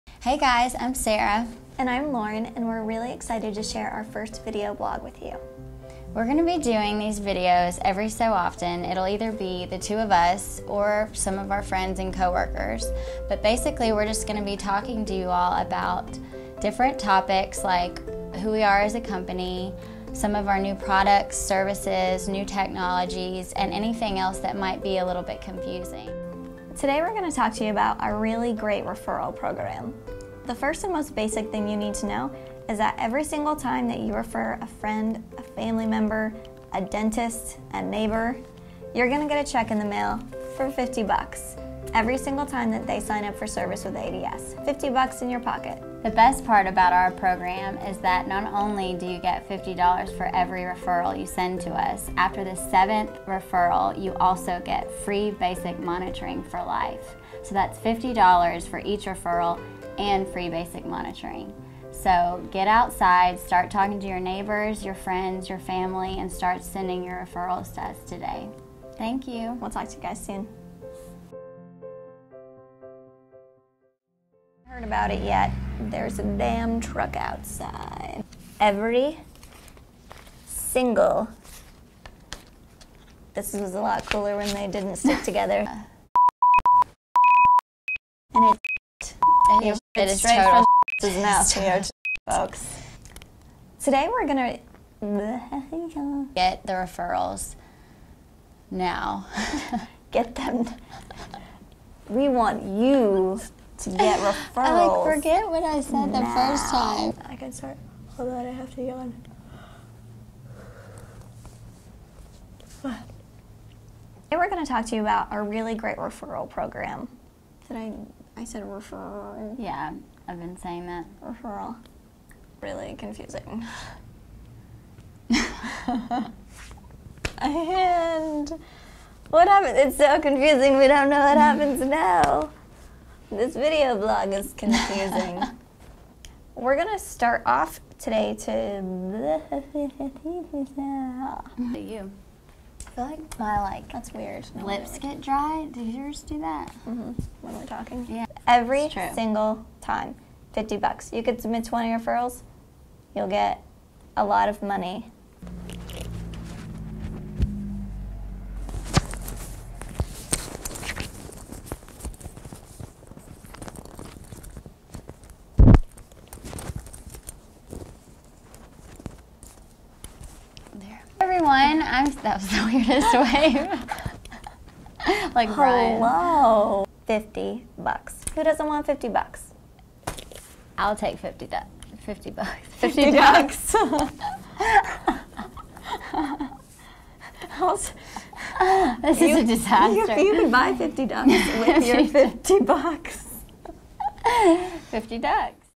ADS Vlog: Referral Program & Bloopers
turns out video blogging is a lot harder than we thought. Learn more about the ADS Referral Program by visiting